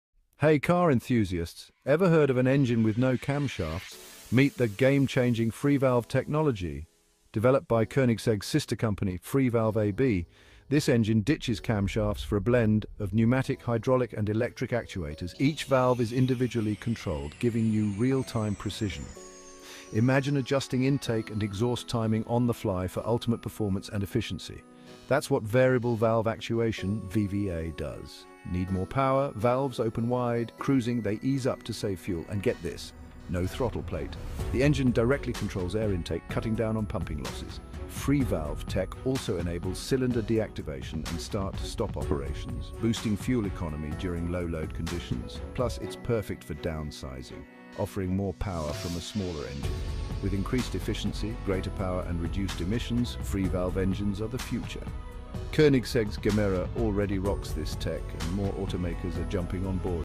Revolutionary Freevalve Engine Explained
Discover the groundbreaking Freevalve technology that is revolutionizing the automotive industry with its camshaft-free design! In this video, we delve into the innovative Variable Valve Actuation (VVA) system, exploring how it optimizes performance and boosts fuel efficiency. Learn about the added benefits of Freevalve, including cylinder deactivation, start/stop operations, and the potential for downsizing engines without compromising power. Don't miss out on this deep dive into the future of automotive engineering!